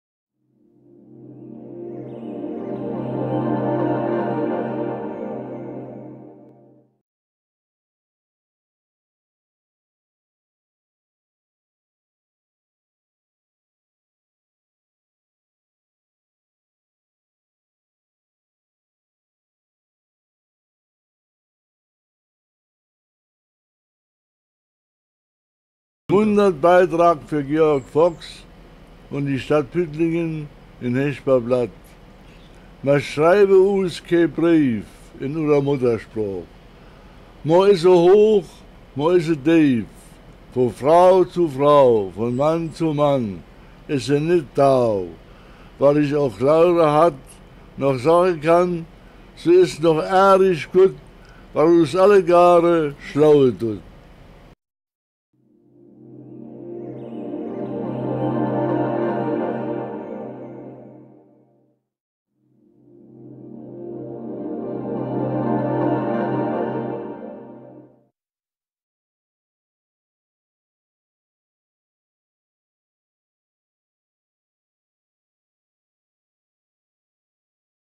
Mundartbeitrag für Literaturweg von Johannes Kühn. Ein Film der Bosener Gruppe
Diesen Beitrag schrieb Johannes Kühn auf Bitten von Georg Fox für den Püttlinger Literaturweg. Prof. Johannes Kühn ist Gründungsmitglied der Bosener Gruppe. Sein literarisches Schaffen ist das Werk eines fleißigen und in den frühen Jahren bisweilen verkannten Dichters. Dadurch, dass er sein Erleben in lyrische Sprache fasst, befreit er sich von Bedrängnissen und schafft eine eigene imaginierte Welt. Das Dorf ist dann nicht mehr das im Saarland gelegene Hasborn/Tholey, sondern es ist das Dorf des Dichters Johannes Kühn. Kühn werden viele hochrangige Preise verliehen. Er ist Kulturpreisträger des Saarlandes und Ehrenbürger seines Heimatortes. Am Dorfbrunnen von Hasborn hat man ihm ein Denkmal errichtet. An den Sitzungen der Bosener Gruppe nahm der Autor seit der Gründung regelmäßig teil.